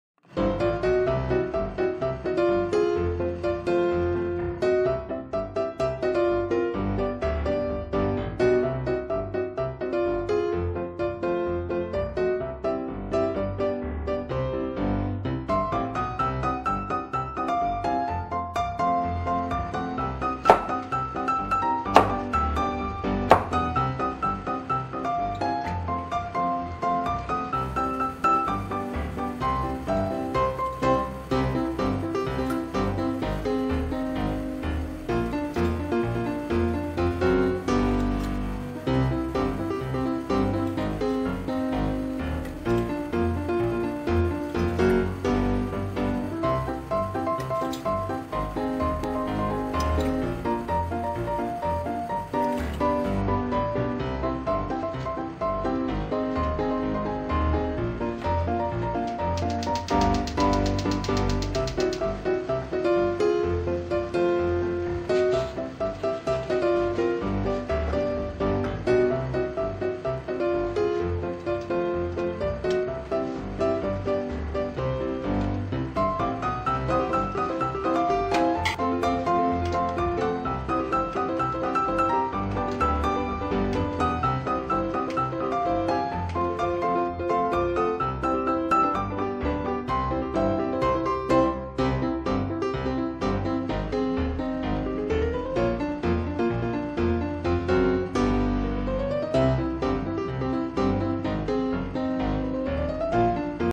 簡単！鯛の煮付け
今回、鯛が手に入ったので煮付けにしてみました。
ごぼうも付け合わせで煮ていますが、お好みで組み合わせてみてください。